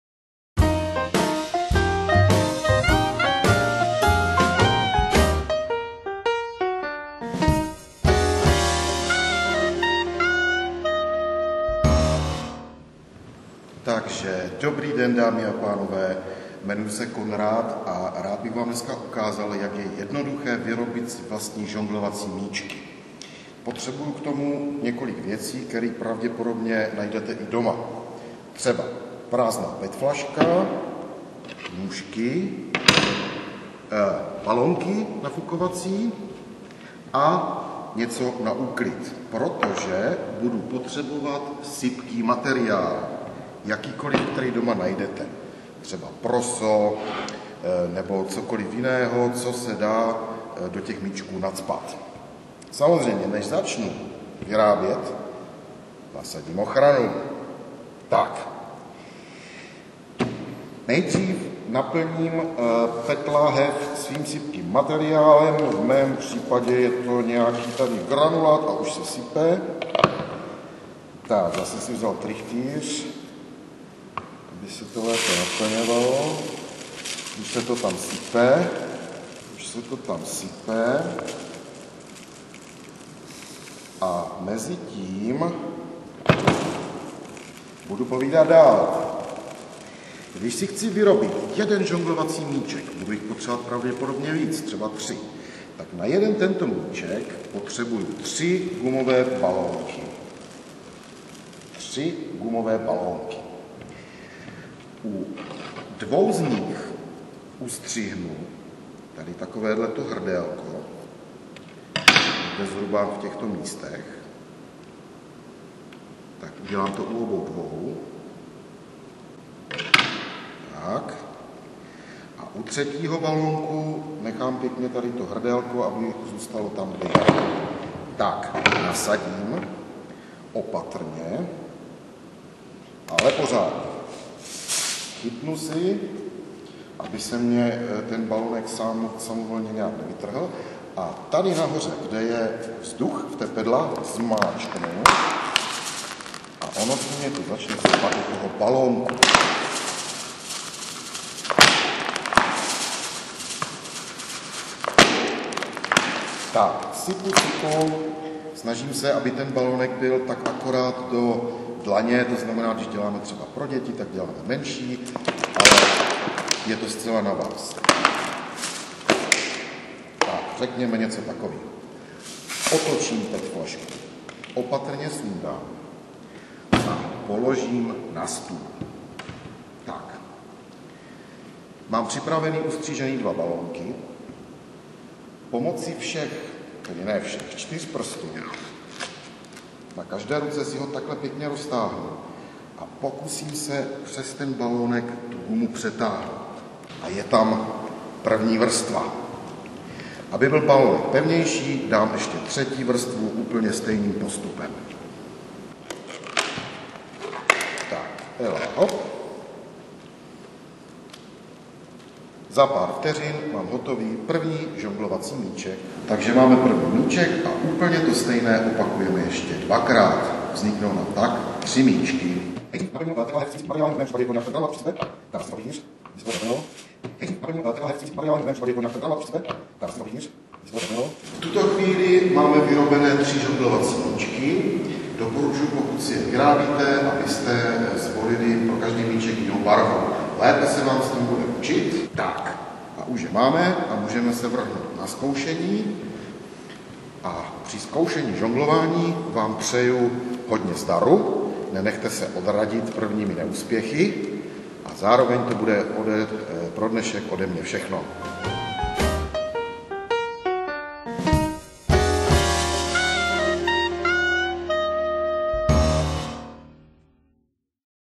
Vyrobte si žonglovací míček! (Legato)
Jste doma s dětmi a už nevíte, co byste mohli podniknout? Chtěli jste se vždycky naučit žonglovat, ale nebyl na to nikdy čas? Začněte teď, splňte si sny a pořádně si u toho zadovádějte! Na výrobu žonglovacích míčků potřebujete jen pár minut a pár věcí, které doma určitě najdete. A pak si s rodinou užijte dechberoucí žonglérskou show. Video návod si pro vás připravili Konrad Heczko a Tadeáš Plch z Cirkusu LeGrando a lužáneckého pracoviště Legato.